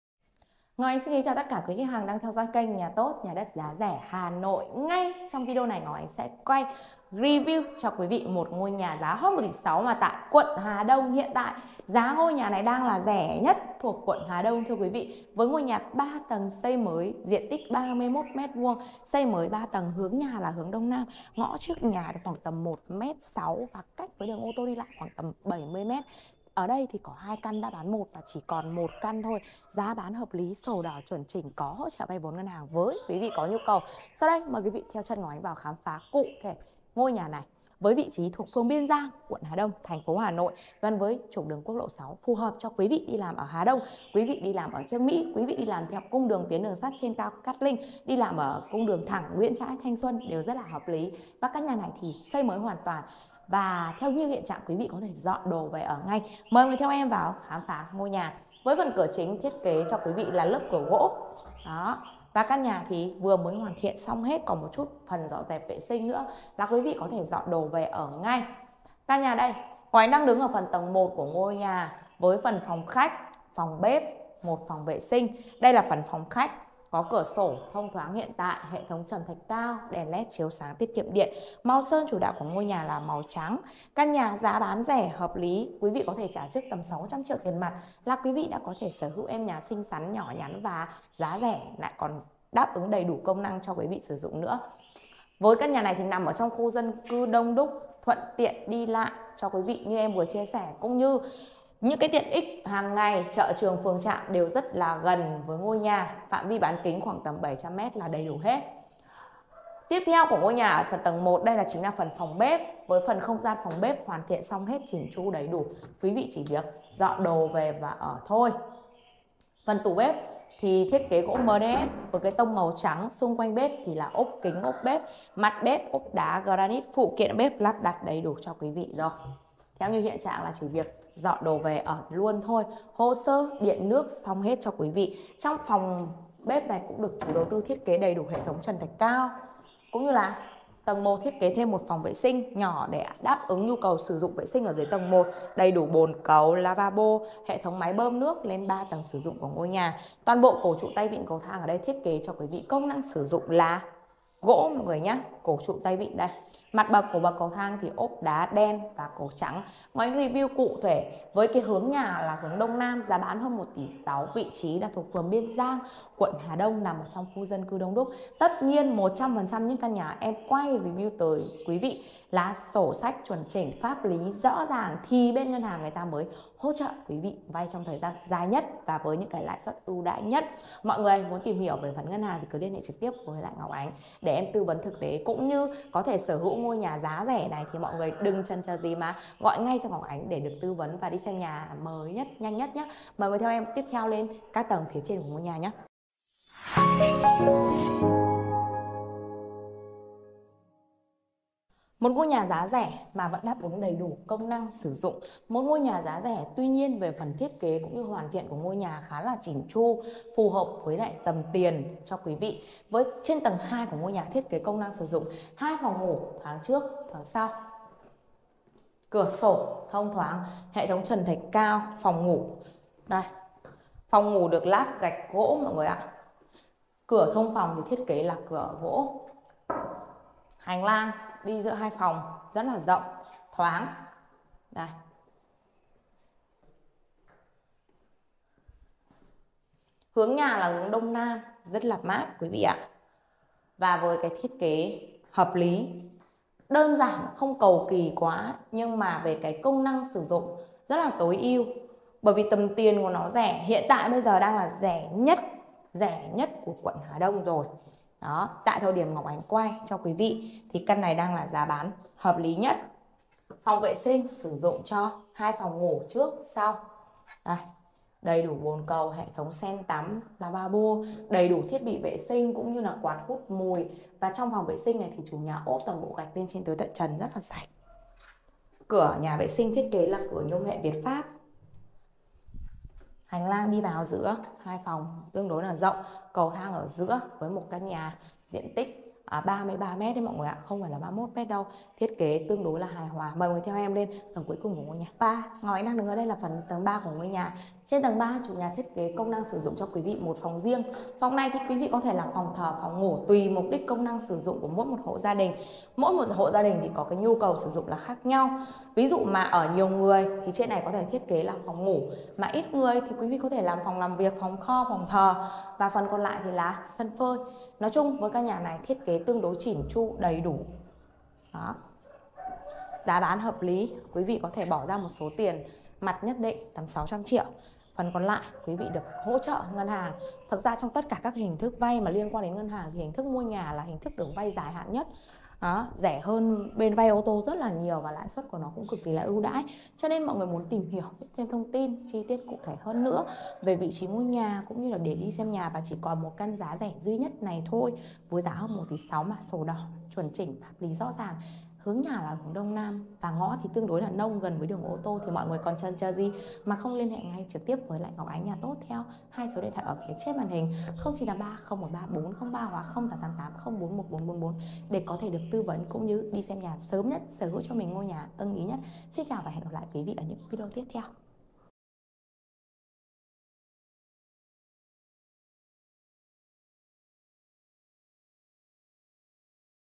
HƠN 1 TỶ 600 TRIỆU - NHÀ QUẬN HÀ ĐÔNG 3 TẦNG, SỔ ĐỎ CHÍNH CHỦ | NHÀ TỐT
- Địa chỉ: Biên Giang, Hà Đông, Hà Nội
- Diện tích: 32m2
- Hướng nhà: Đông Nam
- Thiết kế: 3 tầng, 3PN, phòng khách, bếp, sân phơi
- Giá bán: 1,65 tỷ (có thương lượng)